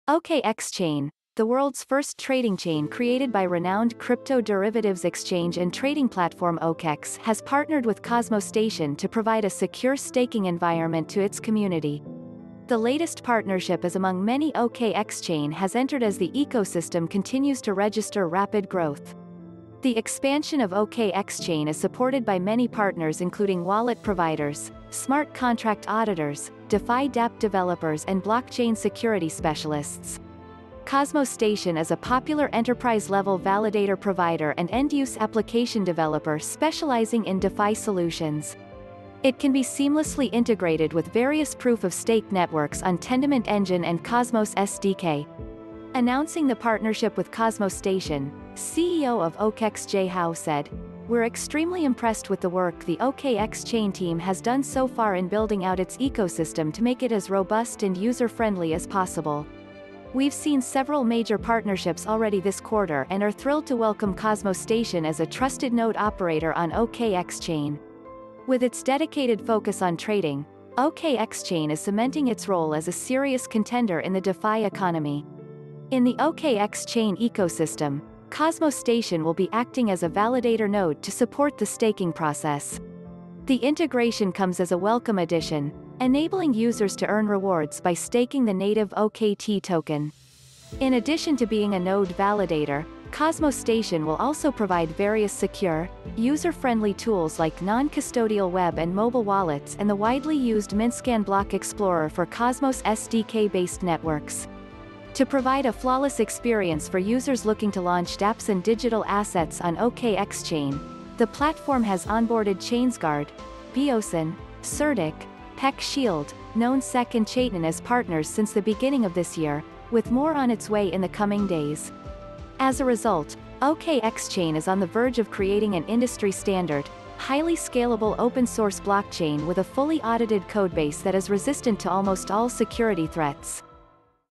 OKExChain Partners with Cosmostation to Enable Secure Staking
OKExChain, the world’s first trading chain created by renowned crypto derivatives exchange and trading platform OKEx has partnered with Cosmostation to provide a secure staking environment to its community. The latest partnership is among many OKExChain has entered as the ecosystem continues to register rapid growth. The expansion of OKExChain is supported by many partners including wallet providers, smart contract auditors, DeFi dApp developers and blockchain security specialists. Cosmostation is a popular enterprise-level validator provider and end-use application developer specializing in DeFi solutions. It can be seamlessly integrated with various proof-of-stake networks on Tendermint Engine and Cosmos SDK. Announcing the partnership with Cosmostation, CEO of OKEx Jay Hao said, “We’re extremely impressed with the work the OKExChain team has done so far in building out its ecosystem to make it as robust and user-friendly as possible. We’ve seen several major partnerships already this quarter and are thrilled to welcome Cosmostation as a trusted node operator on OKExChain. With its dedicated focus on trading, OKExChain is cementing its role as a serious contender in the DeFi economy.”In the OKExChain ecosystem, Cosmostation will be acting as a validator node to support the staking process. The integration comes as a welcome addition, enabling users to earn rewards by staking the native OKT token. In addition to being a node validator, Cosmostation will also provide various secure, user-friendly tools like non-custodial web and mobile wallets and the widely used Mintscan Block Explorer for Cosmos-SDK-based networks. To provide a flawless experience for users looking to launch dApps and digital assets on OKExChain, the platform has onboarded Chains Guard, Beosin, Certik, PeckShield, KnownSec and Chaitin as partners since the beginning of this year, with more on its way in the coming days. As a result, OKExChain is on the verge of creating an industry-standard, highly scalable open-source blockchain with a fully audited codebase that is resistant to almost all security threats.